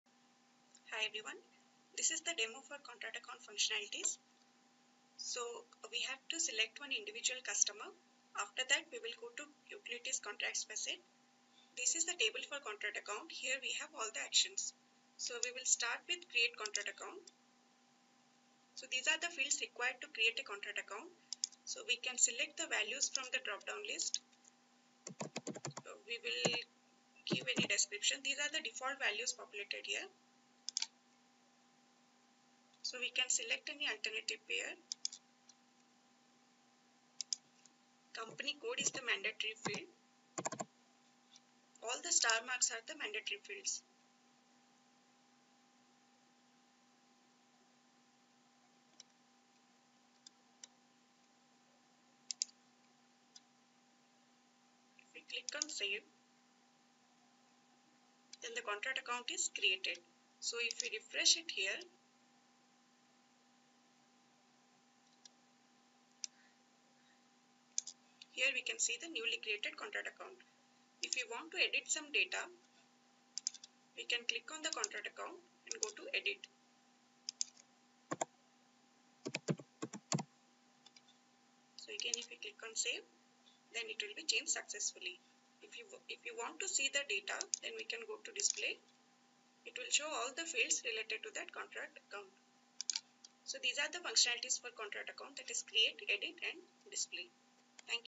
SAP C4C Utilities Contract Account
This is a how to guide for Create, update, delete scenario on Contract Account for the Customer for Utilities Industry.
It is a helper content only and not a official document from SAP.